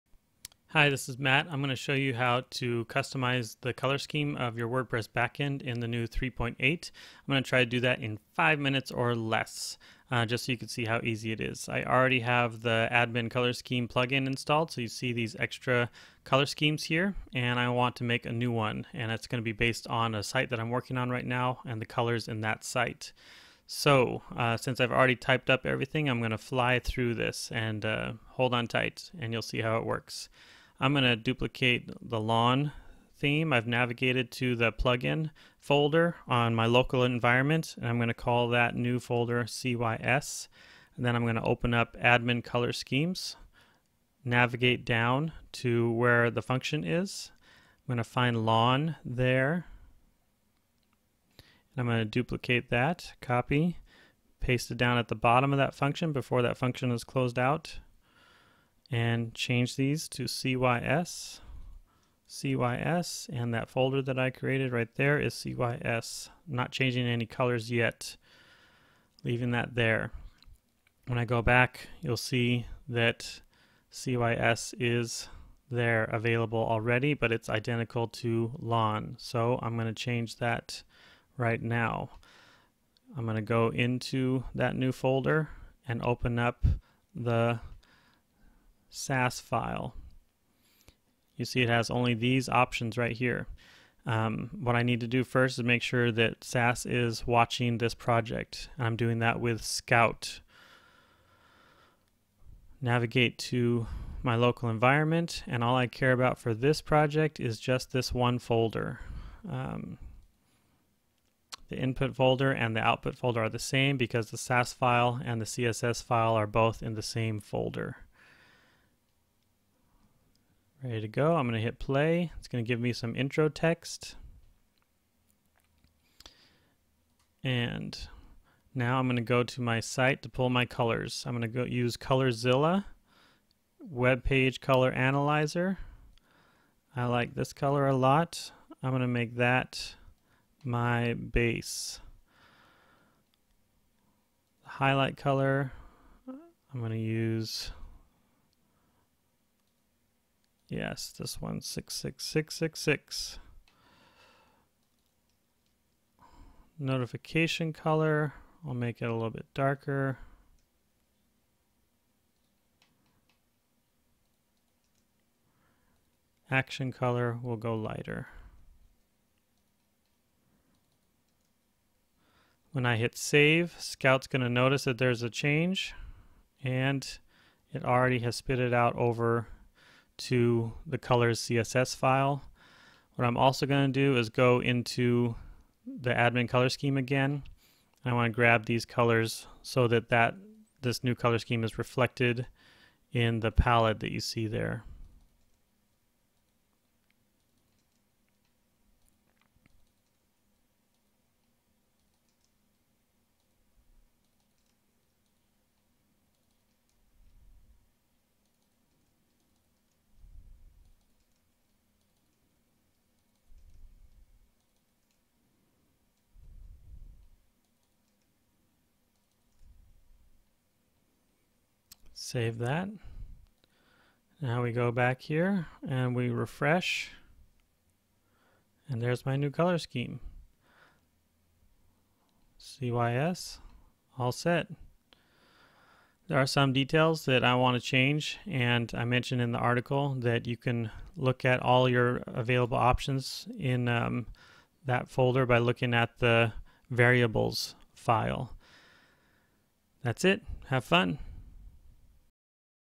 How to Create a Custom Admin Color Scheme in WordPress 3.8
This is really just an overview. I detail it all in the full article